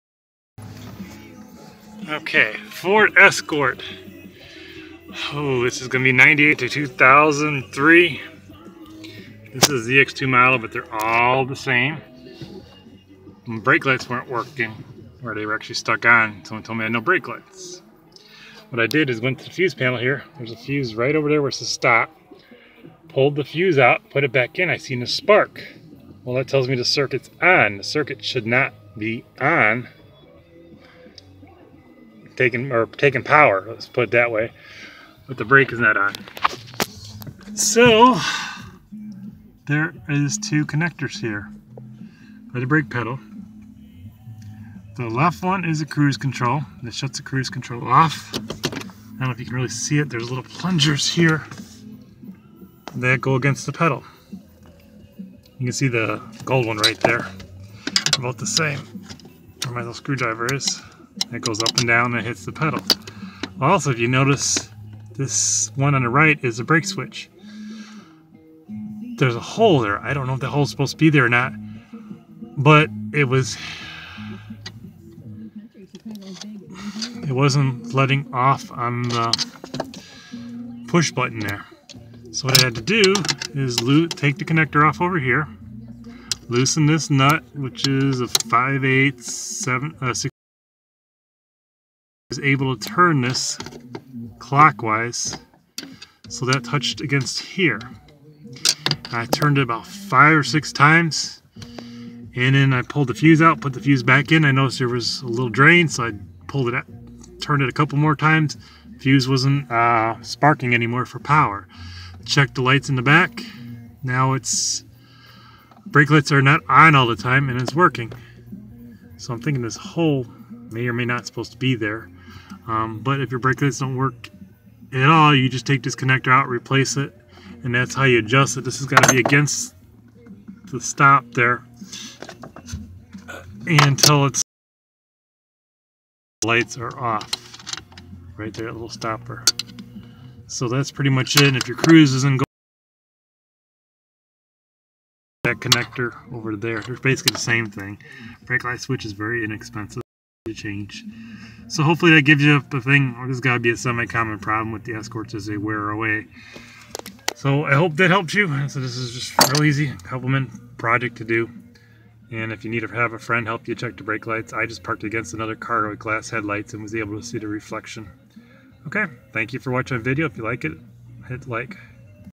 98-03 Ford Escort ZX2 brake light switch
Fixing brake lights / tail lights on Ford Escort ZX2 due to stuck on brake lights. This appears to been needed to be adjust cause of a rubber pad worn out or gone. Affects cruise control also.